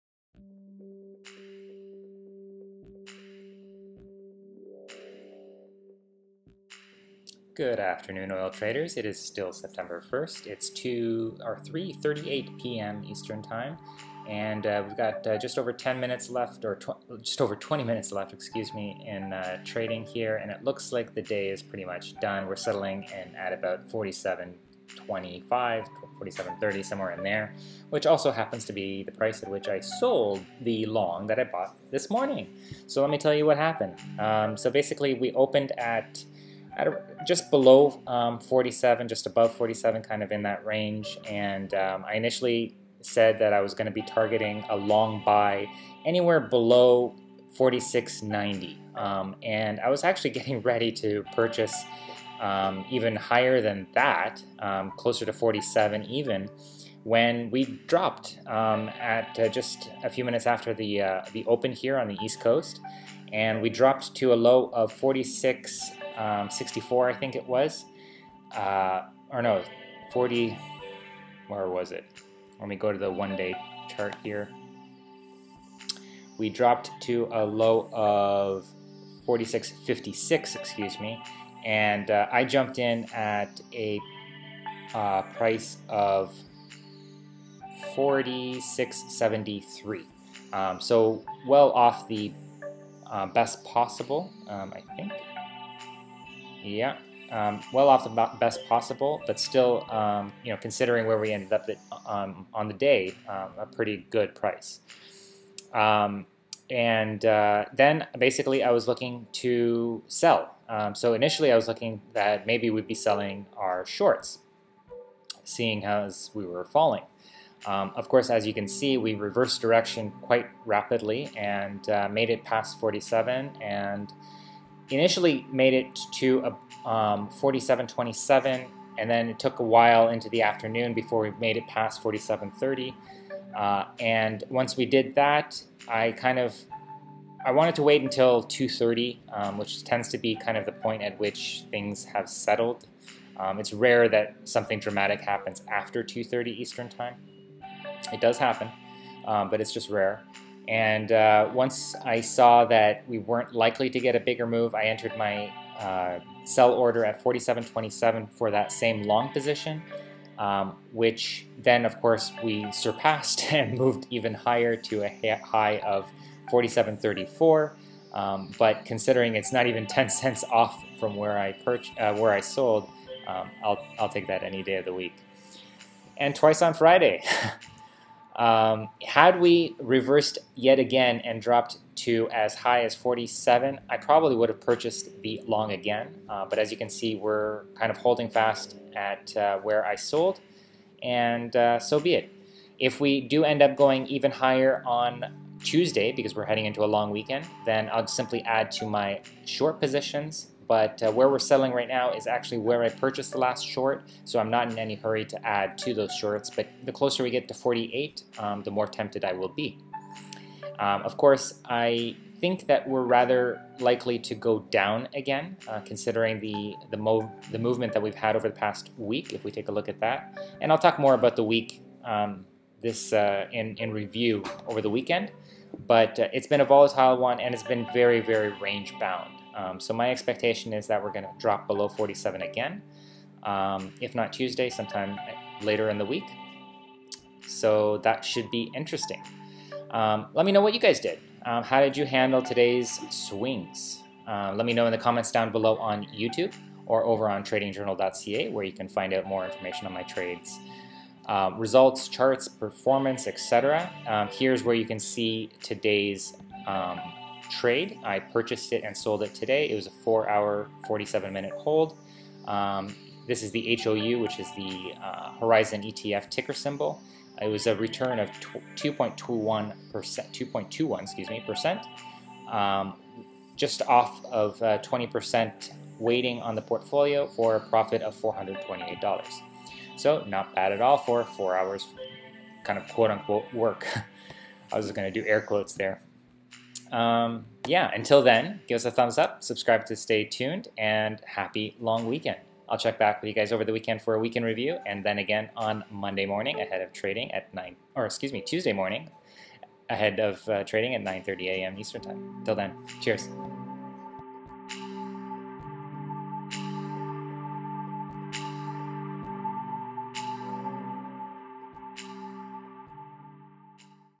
118 - Results for Sept 1, 2017 - Bought Long as Hedge on Morning Drop & Sold for a $428 Profit
* Stuck to the plan and bought long near the open when we dropped - bought HOU at a Crude price of $46.73
    * I was initially tempted to sell yesterday’s short buy from $47.25 at this price too (+2%), but decided to wait for Europe
* I waited to 2:30p EST, but decided to take a profit on the long today at $47.28
    * Crude tried to climb higher, but ultimately settled
    * I would have repurchased the long had we dropped to $47
* Happy long weekend all!